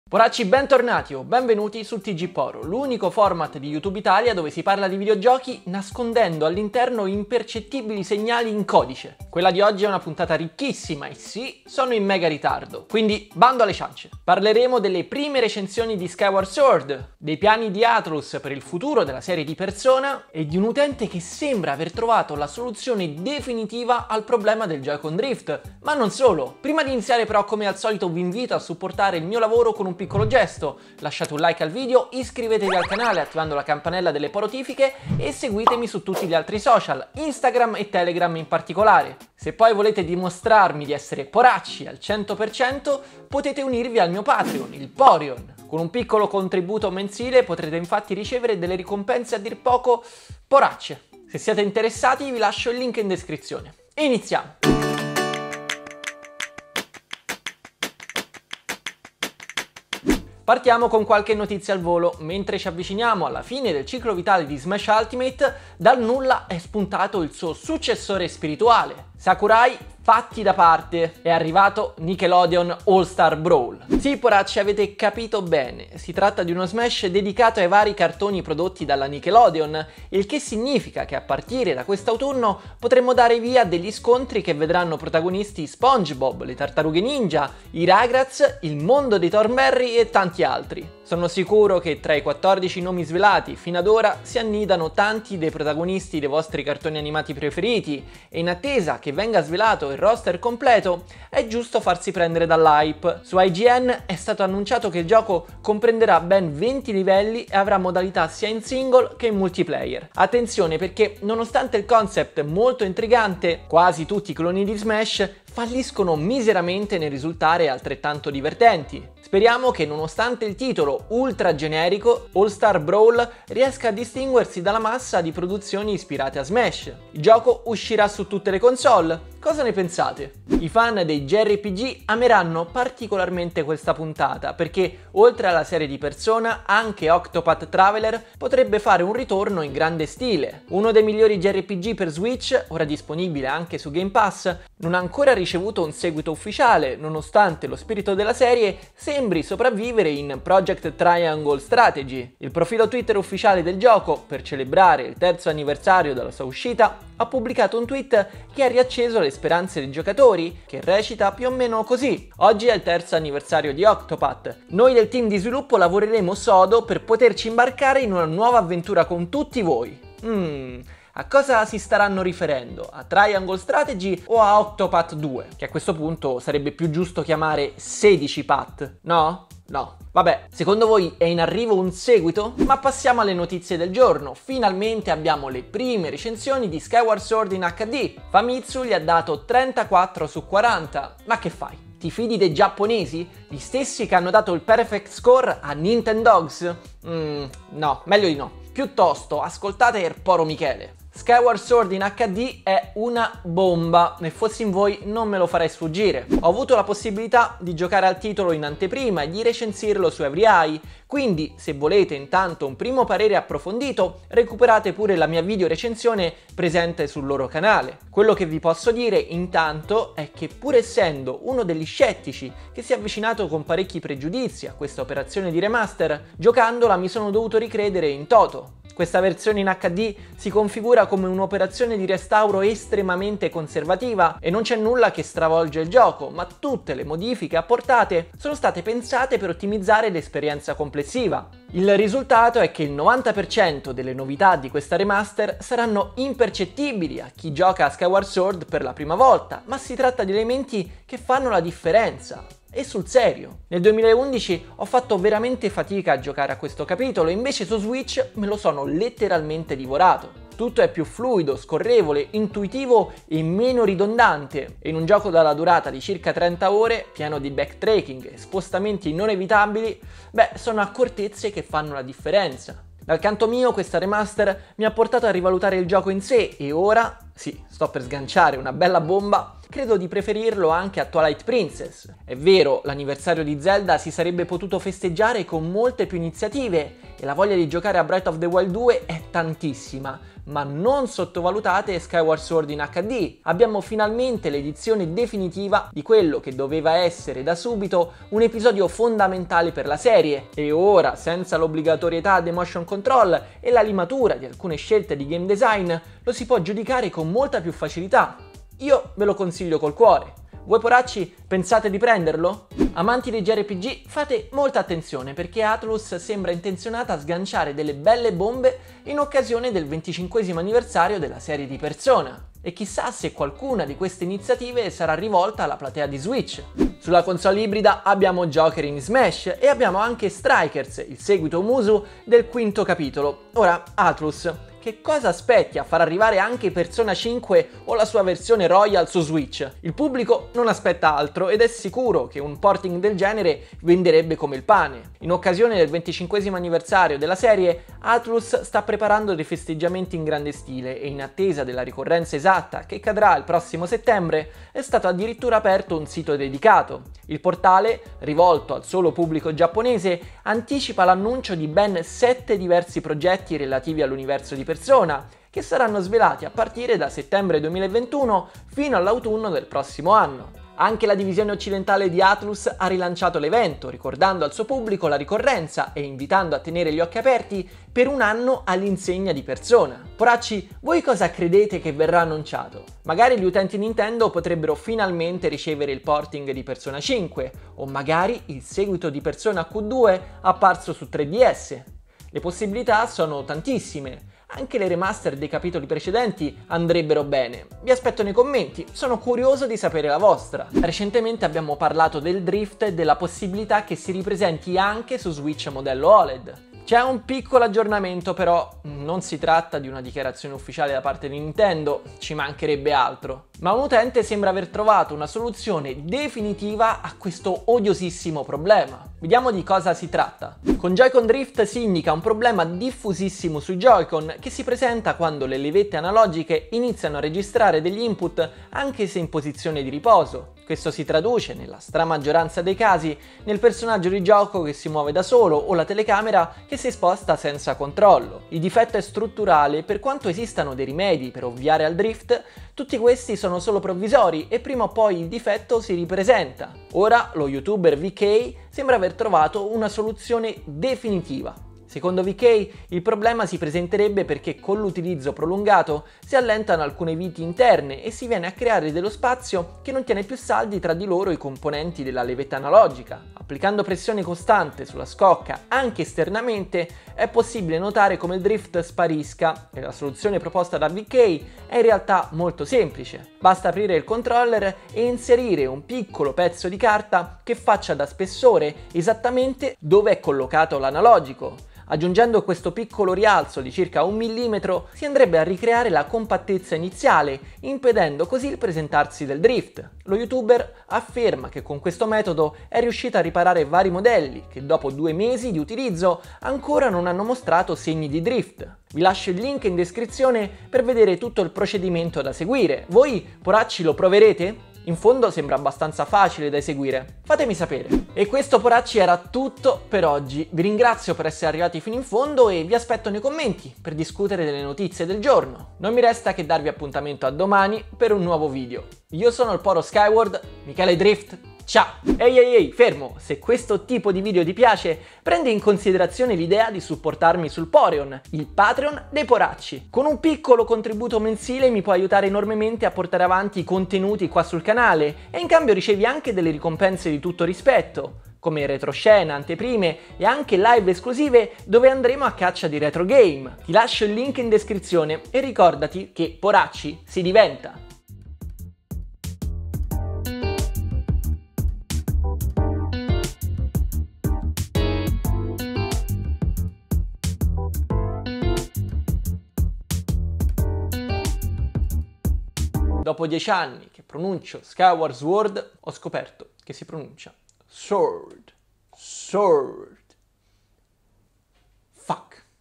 VALE LA PENA COMPRARE SKYWARD SWORD HD? NOVITÀ PER PERSONA E TROVATA LA SOLUZIONE AL JOYCON DRIFT!
Poracci bentornati sul TG PORO!
Oggi parleremo delle prime recensioni di Skyward Sword HD. Vale la pena comprare la remaster di un gioco Wii venduta a prezzo pieno? Avendo giocato e finito la versione HD posso dirvi che sì, vale la pena acquistare Skyward Sword su Switch! Questa versione in HD sistema tutti i problemi dell'originale e dona una nuova vita a Skyward Sword, un capitolo da provare assolutamente se si è fan della saga di The Legend of Zelda!
Atlus stuzzica i fan della serie Persona annunciando 7 nuovi progetti per celebrare il 25simo anniversario della serie. Possiamo aspettarci qualche arrivo importante anche su Switch? Vedremo Persona 5 Royal su console ibrida? E Persona 6?
VK, uno youtuber, afferma di aver trovato la soluzione DEFINITIVA al joycon drift. VK spiega in video un procedimento molto semplice per sistemare in modo permanente le levette dei controller Switch. Cosa ne pensate? Secondo voi funziona?
Vi aspetto nei commenti!

00:00 Introduzione
01:01 Nickelodeon All-Star Brawl: ecco Smash con Spongebob e le Tartarughe Ninja!
02:11 Octopath Traveler: Square Enix lavora al seguito?
03:03 Skyward Sword HD, la recensione. Vale la pena acquistare questo Zelda su Switch?
05:13 Atlus festeggia Persona: 7 nuovi progetti per il 25simo anniversario della serie
06:41 Joycon drift: Uno youtuber ha trovato la soluzione DEFINITIVA?
08:28 Outro e Patreon